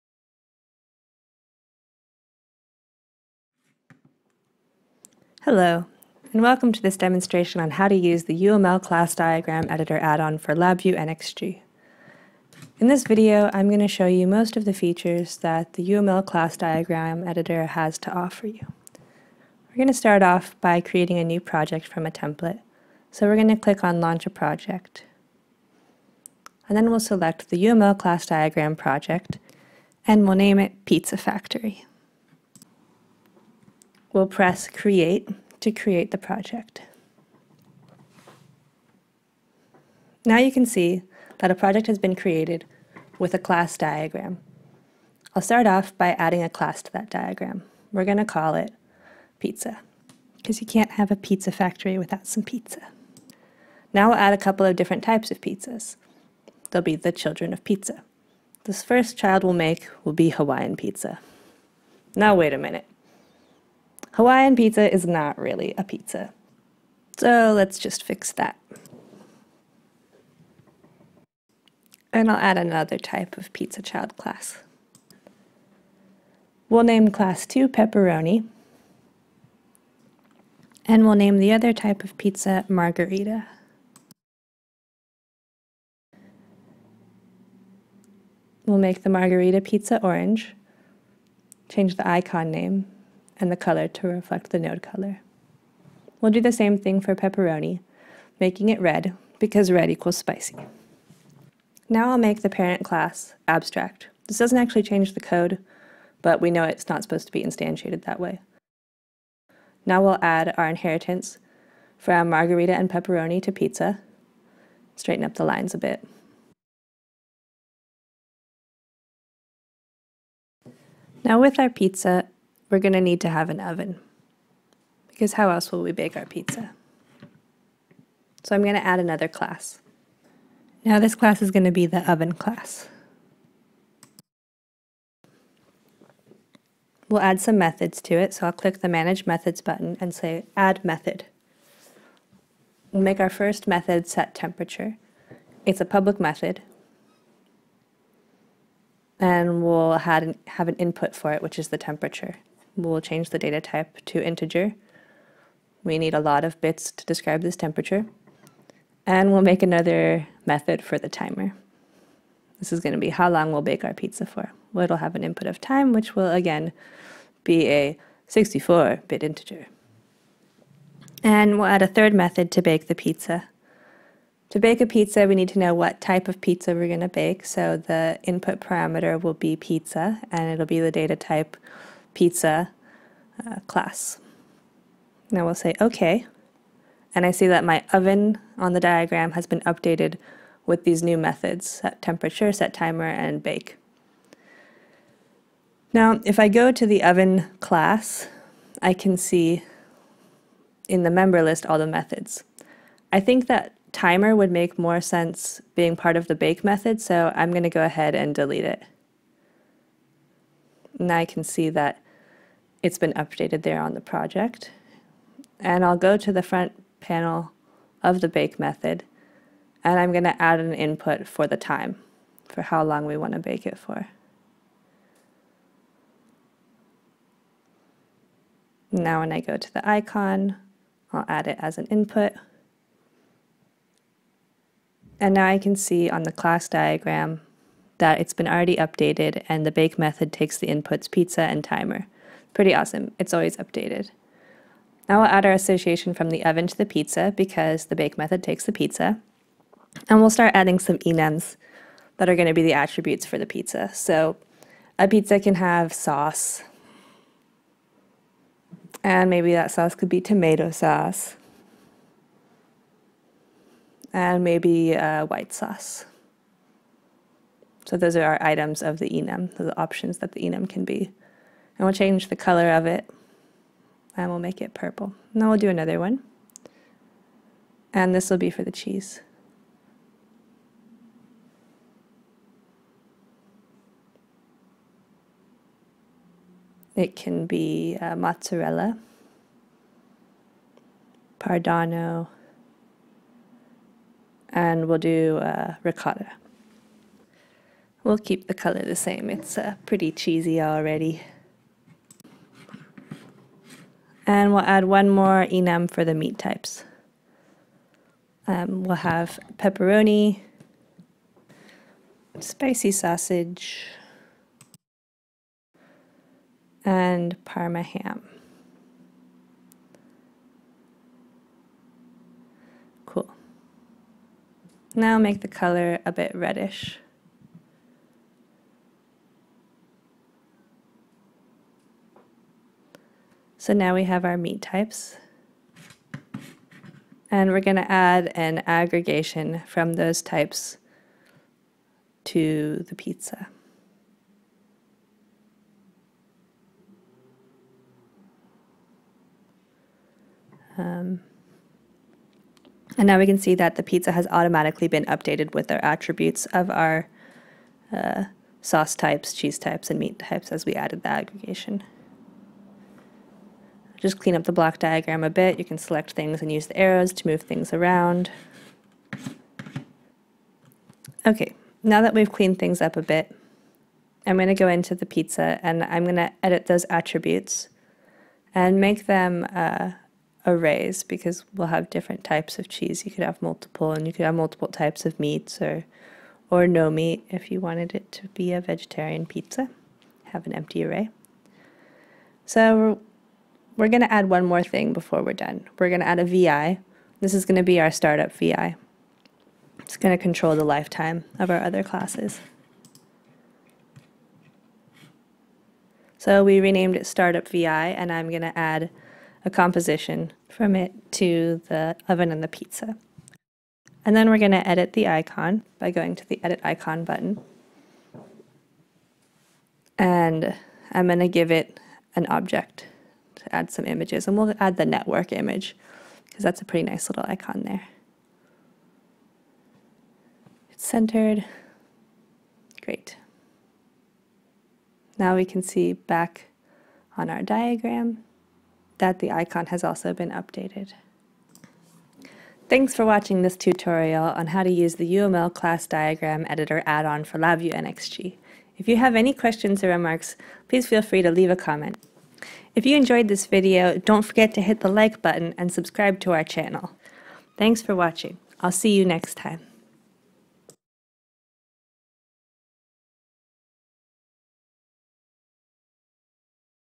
UML Class Editor Add-on for LabVIEW NXG - Create your first class diagram
Watch this short tutorial to find out how easy peasy it is to create a class diagram in LabVIEW NXG!
Watch all of the boiler-plate code be created for you on the fly and how many actions you save compared to doing it all manually.

Editor:
Bas v. Etten